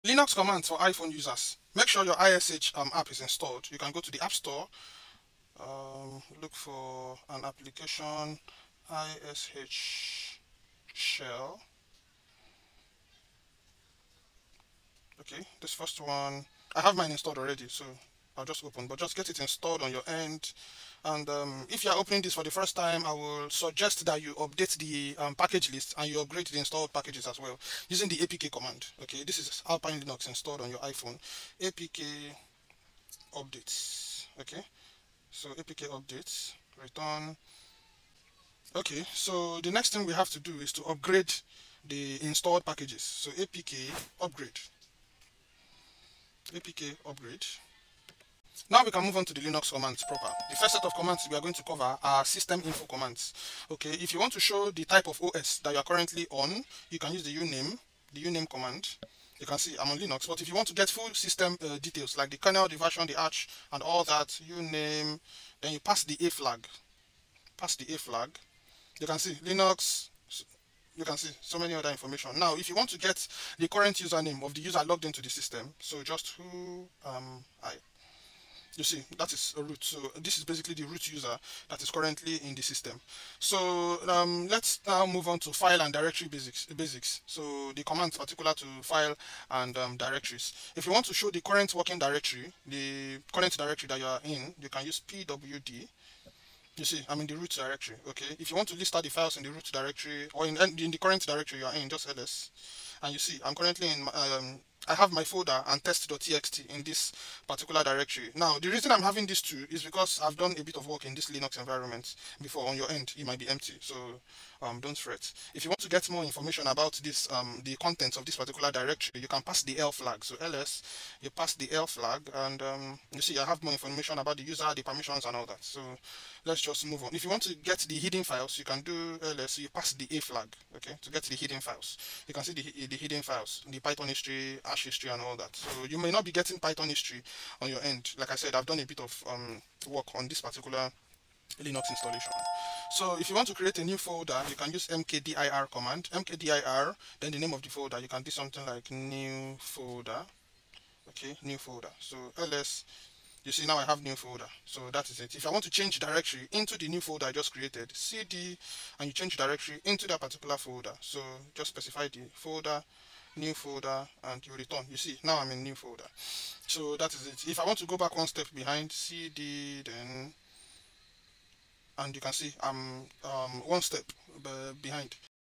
💻 Linux Terminal on iPhone? YES! Essential iSH Shell Commands in 3 minutes
🚀 Run REAL Linux commands on your iPhone/iPad! This Short teaches you must-know terminal skills using iSH Shell:
✔ uname, whoami, pwd (System Info)
✔ ls, mkdir, cd (Navigation)

💡 iOS Pro Tip: Pinch-to-zoom for better readability!
🔔 Follow for more iOS developer tools